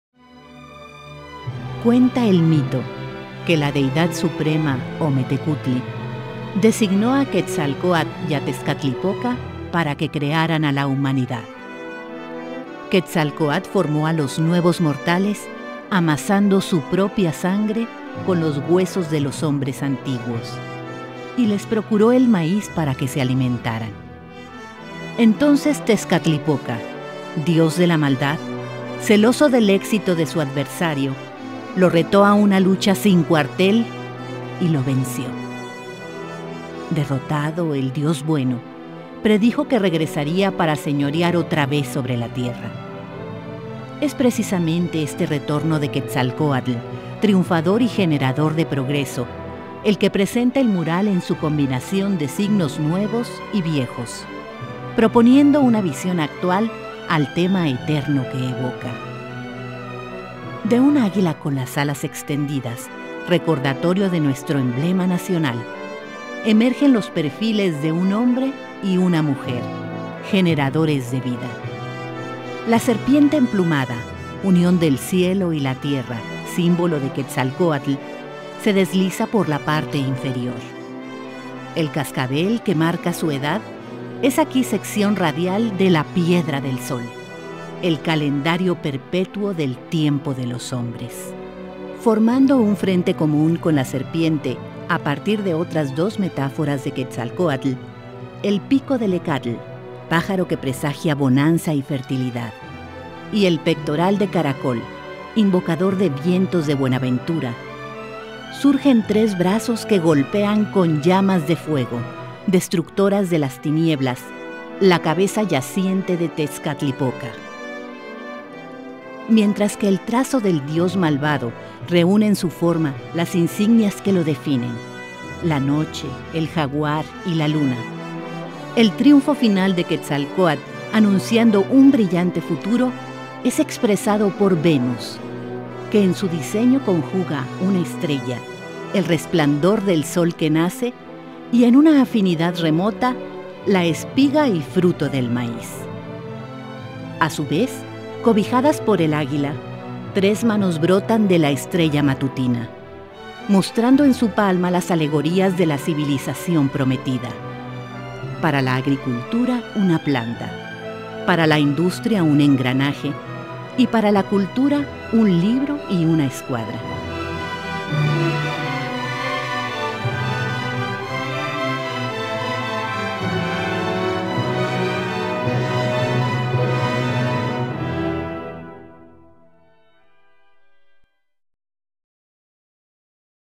Significado del Mural del Tecnológico de Monterrey
El Mural de Rectoría del Tecnológico de Monterrey es un alto relieve tallado en piedra y aderezado con mosaico; representa el triunfo diario de la civilización y la cultura sobre las fuerzas oscuras del estancamiento y la apatía. Para ejemplificarlo, Jorge González Camarena eligió la lucha y victoria de Quetzalcóatl sobre Tezcatlipoca.

Por el talento concentrado en sus siete metros de alto por 32.40 de largo, el simbolismo del Mural de Rectoría es hoy tan válido como en el momento de su develación, en 1954.